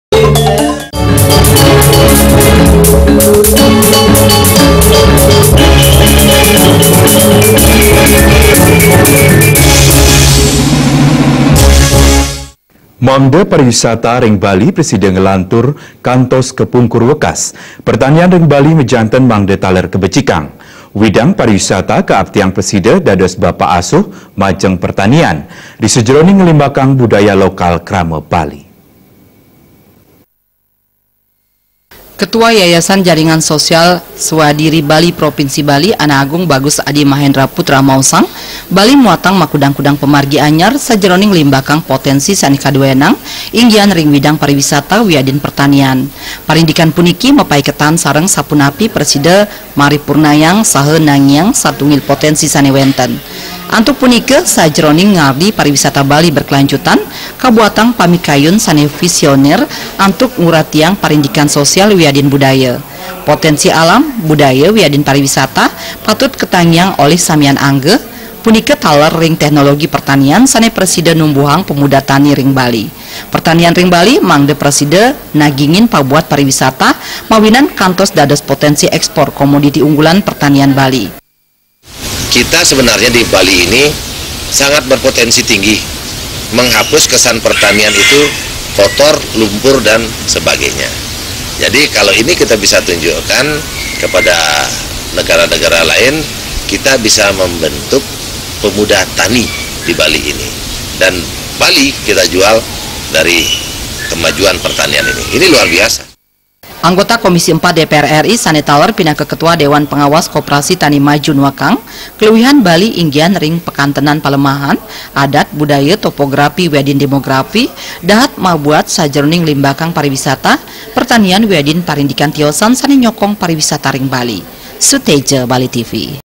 NANGIANG PERTANIAN RING BALI
MANGDA PARIWISATA BALI PRASIDA NGELANTUR KANTOS KAPUNGKUR WEKAS, PERTANIAN RING BALI MAJANTEN MANGDA TALER KABECIKANG. WIDANG PARIWISATA KAAPTIYANG PRASIDA DADOS BAPAK ASUH MAJENG PERTANIAN, RISAJRONING NGELIMBAKANG BUDAYA LOKAL KRAMA BALI.